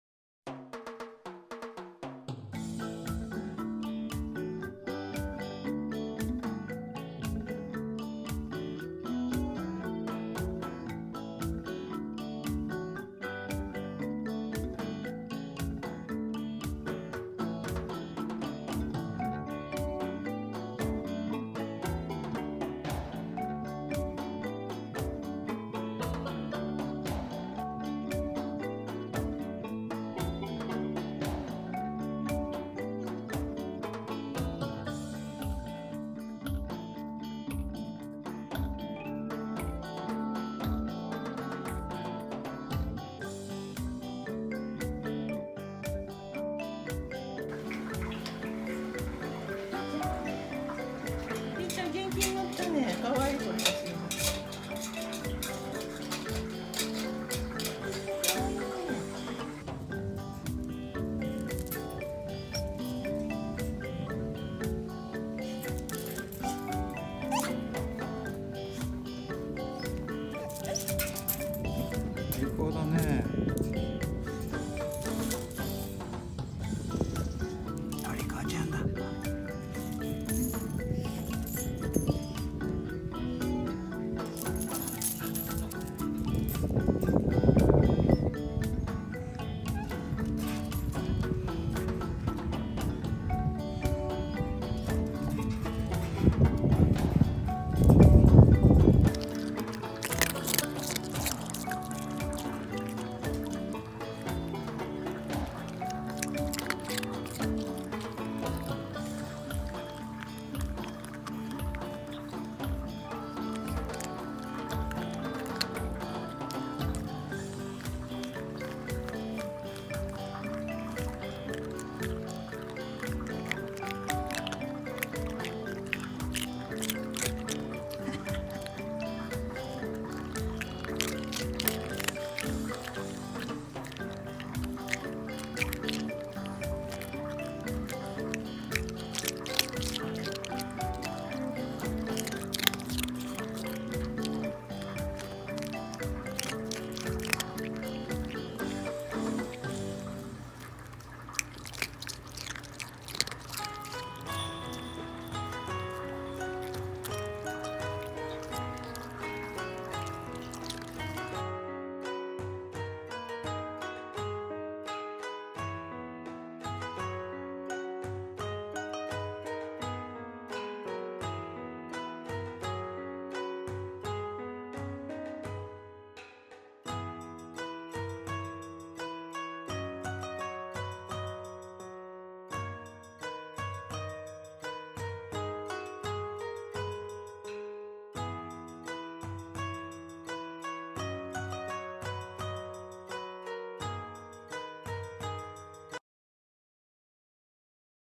小夏、豆柴やめるってよ！小春
ポッチャリな小夏さんをお楽しみ下さい！
小春（母）13歳
小夏、陸、小豆、空　11歳
比嘉さん　13歳（腸内フローラ年齢）